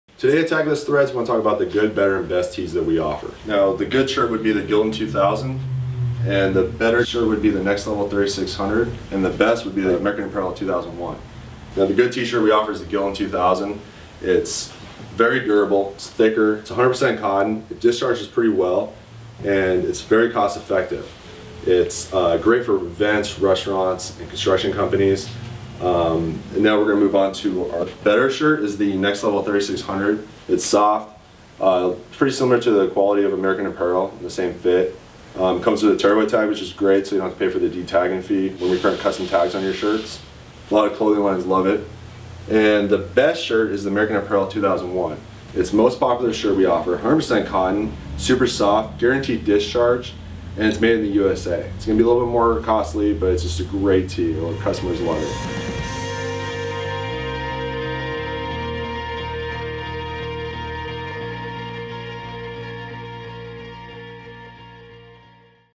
Good, Better, Best, T-shirts by Tagless Threads Customers
Good, Better, Best, T-shirts as voted by Tagless Threads Customers. The good T-shirt is the durable and cost effective Gildan (Style Number 2000). The better t-shirt is the Next Level (Style Number 3600) which is popular among fashion lines as a result of it's slim fit style and tear away tag. The best T-shirt is the American Apparel (Style Number 2001) which also is popular with fashion lines and looks flawless when screen printed with discharge and water based ink.